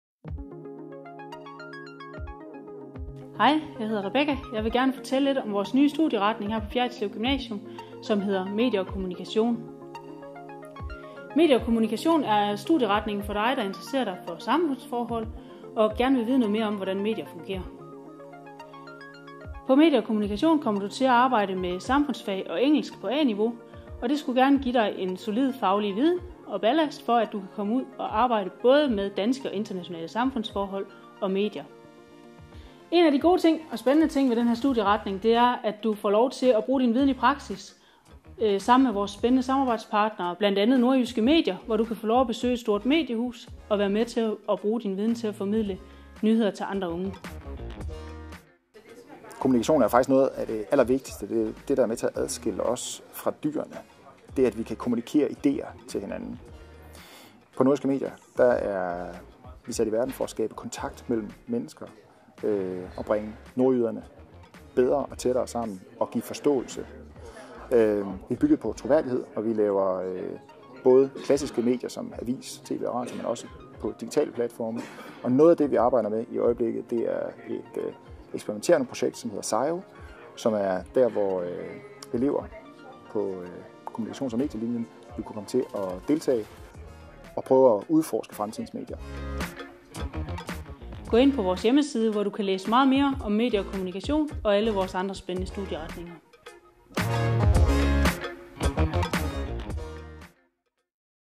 Medier & Kommunikation på Fjerritslev Gymnasium
Medier & Kommunikation er en ny studieretning på Fjerritslev Gymnasium

Studieretningen er for dig, der har lyst til at beskæftige dig med politik, økonomi og sociologi – og som er nysgerrig efter at vide, hvordan medier fungerer